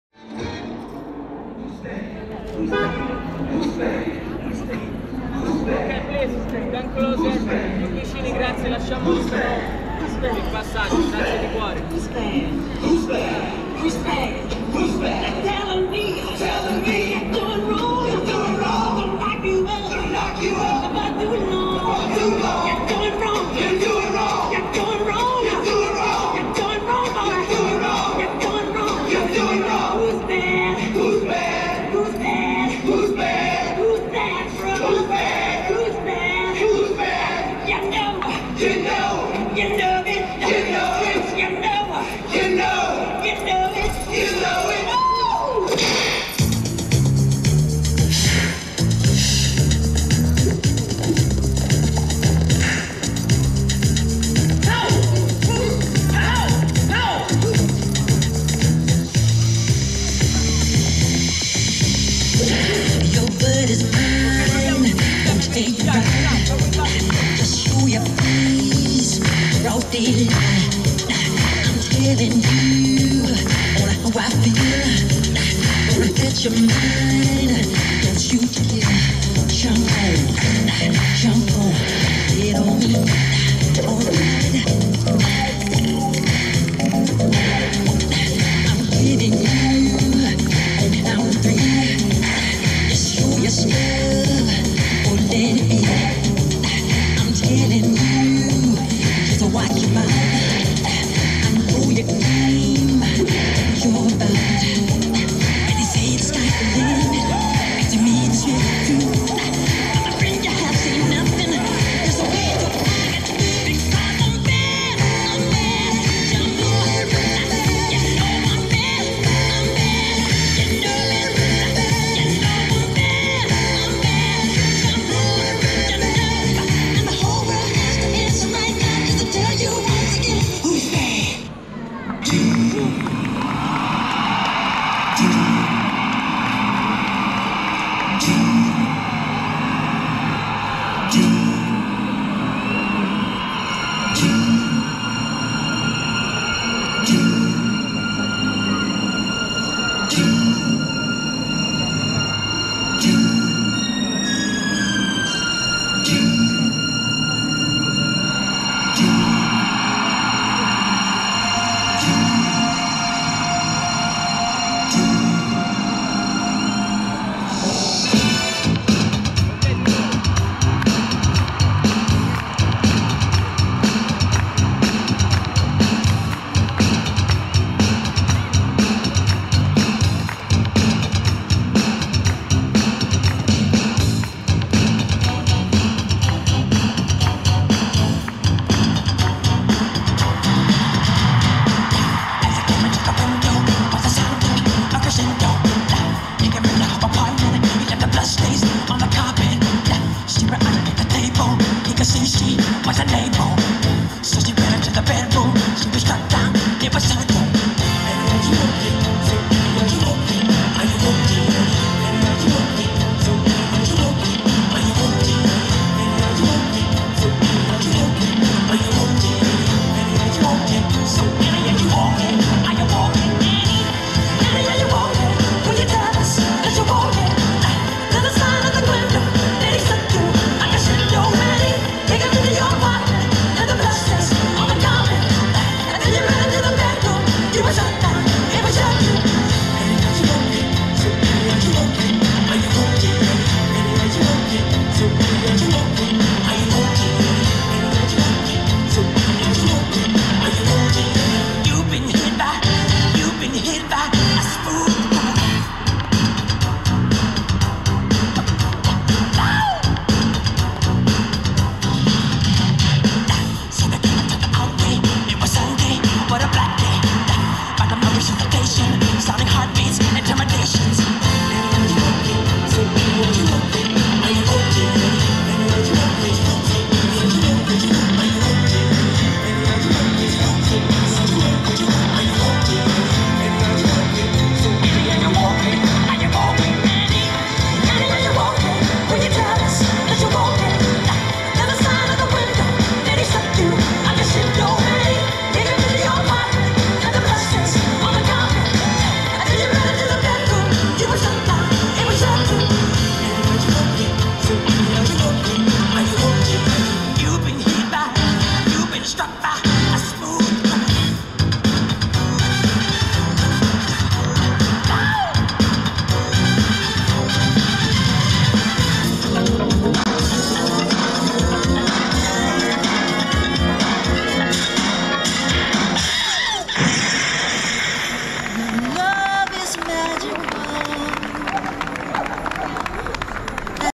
Amazing Michael Jackson impersonator street performance in Rome, Italy 4K
Michael Jackson impersonator in Rome, Italy! One of the best Michael Jackson  impersonator in Rome! A cool street performance. A talented guy dancing like Michael Jackson.